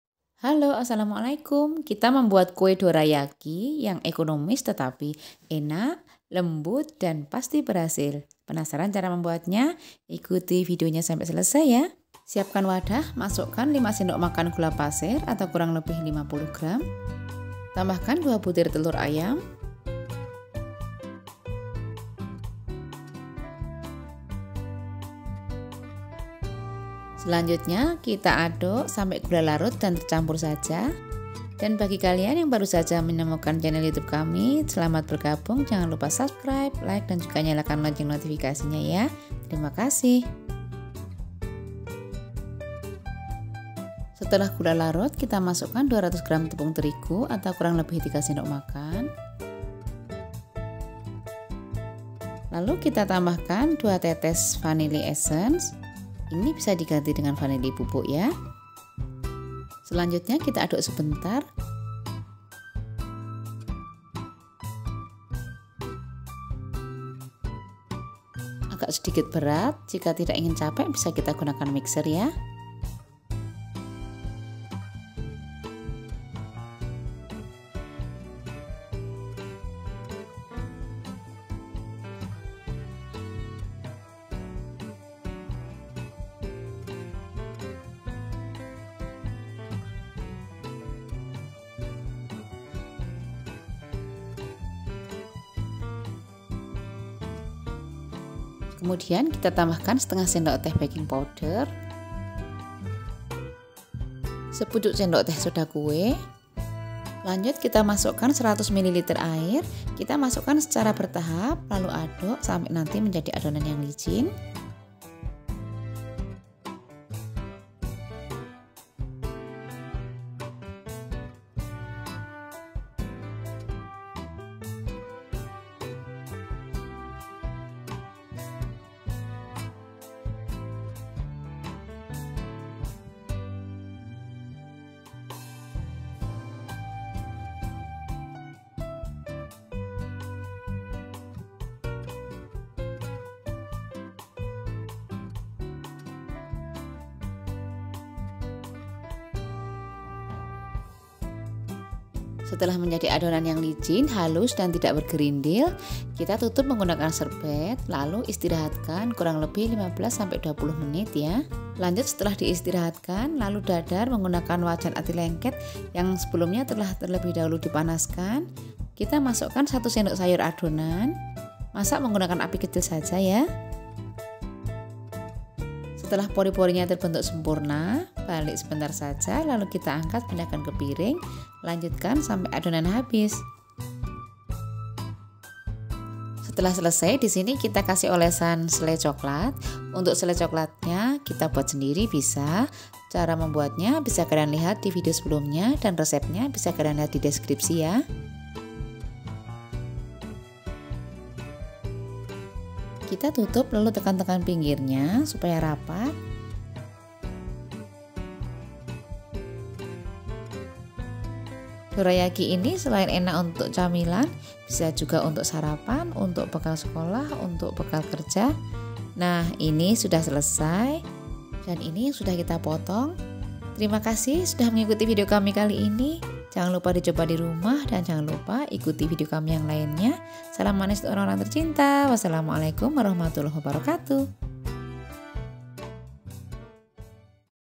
Dorayaki teflon | Ekonomis, Empuk dan Enak
Penasaran dengan Cara membuat kue Dorayaki kue yang disukai doraemon itu?
Ikuti video cara membuatnya.
Meski Ekonomis dan hanya menggunakan teflon, tapi beneran dapat menghasilkan Kue Dorayaki yang empuk dan enak.